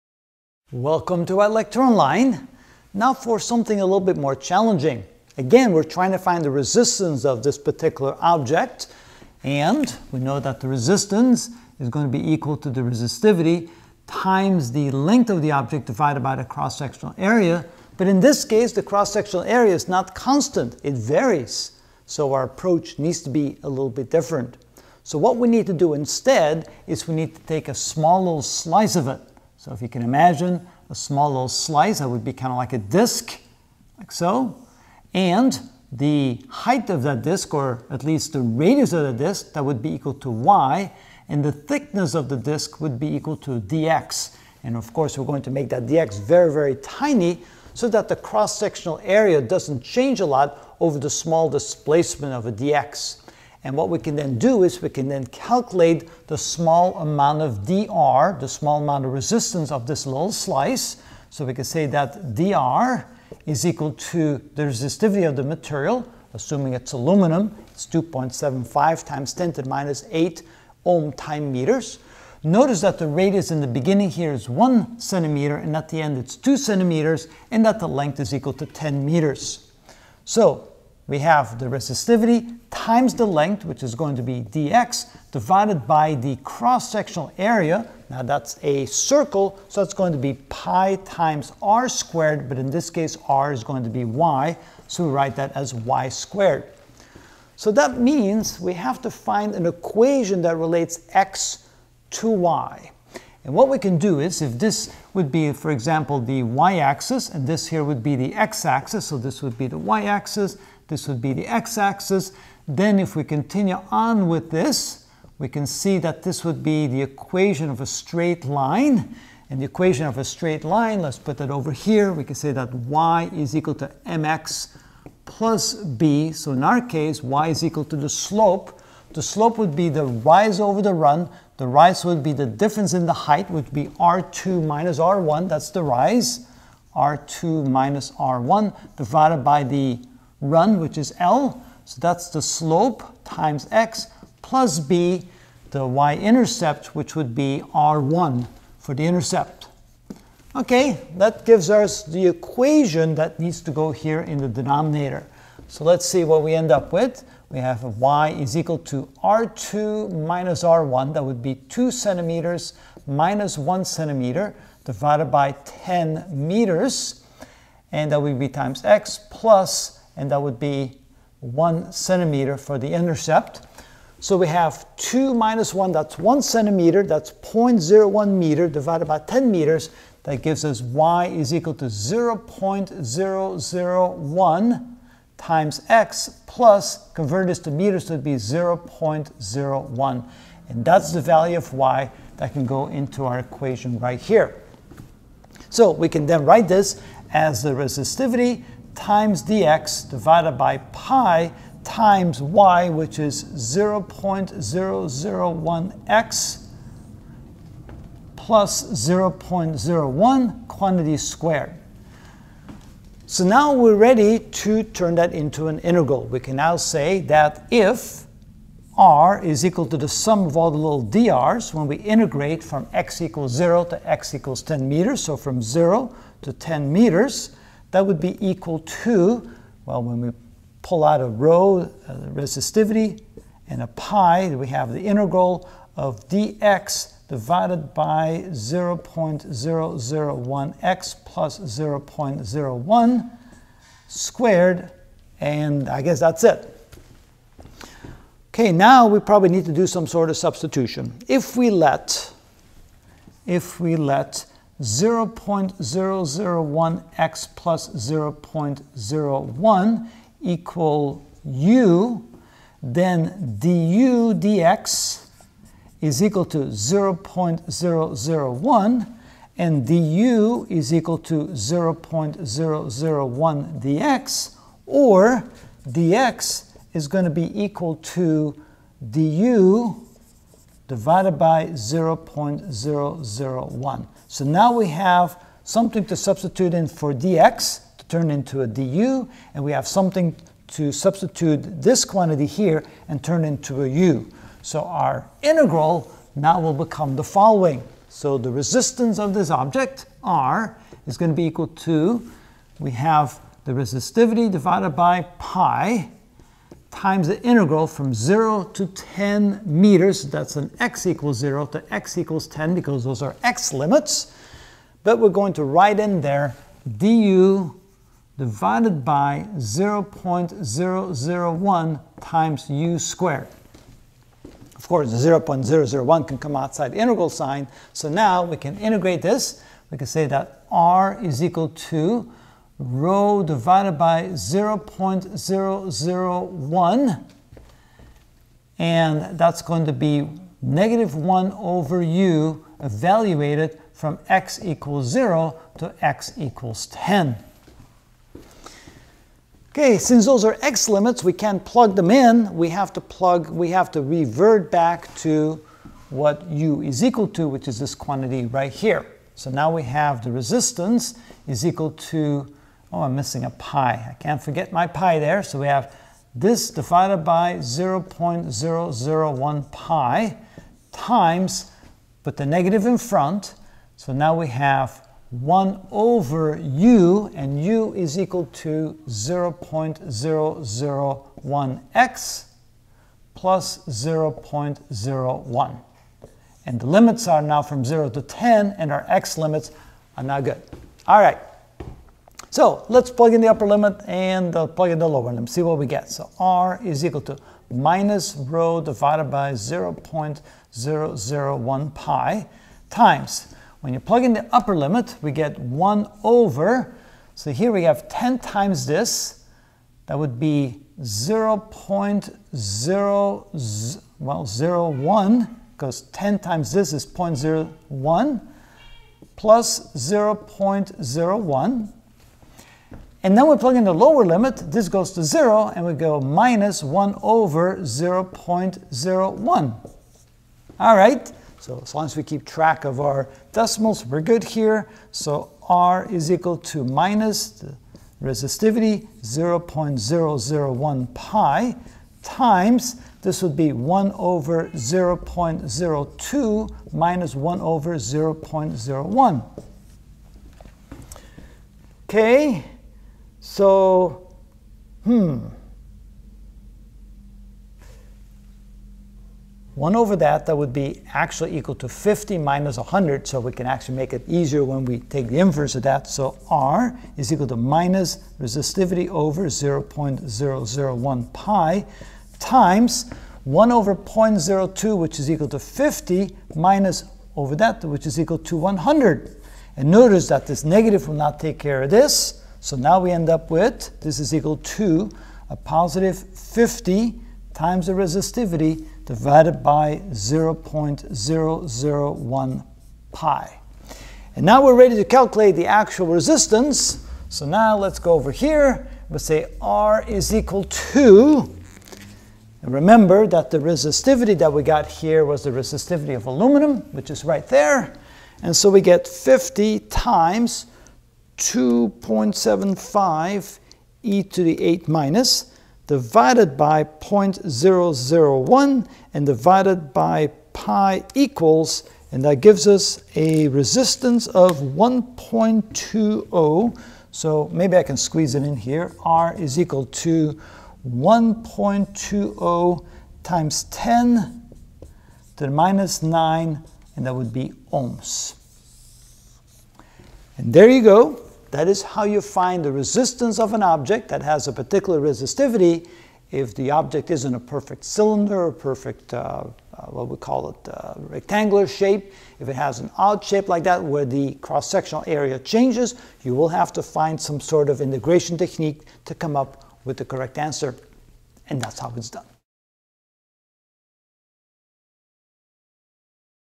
Physics - E&M: Ch 40.1 Current & Resistance Understood (13 of 17) Resistance=? & Resistivity=? Ex.2
In this video I will find the resistance, R=?, of a truncated cone shape aluminum of length L=10m with radii R1=1cm and R2=2cm and a given resistivity.